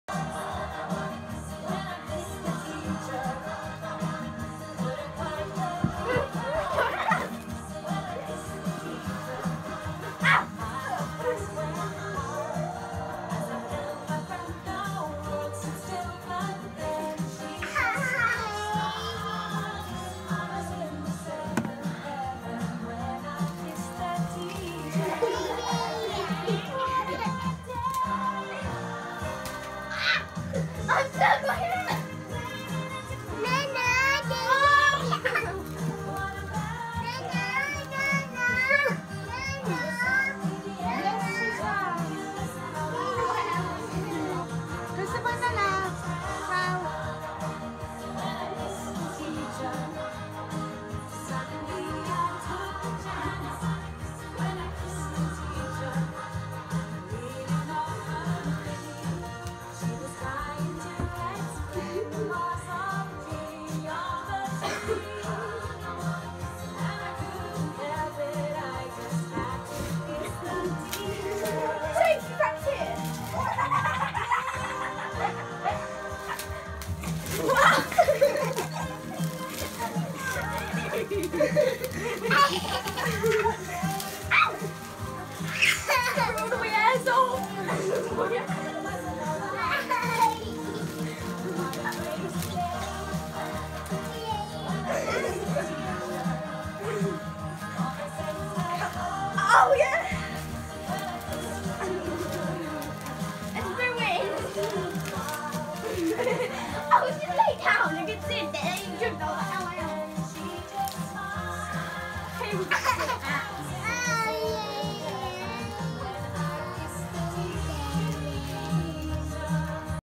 Spider web maze fun
Cling film a spiders 🕷 web with sellotape for catching those little bugs!   Daisy and Sophie the baldwins at it again!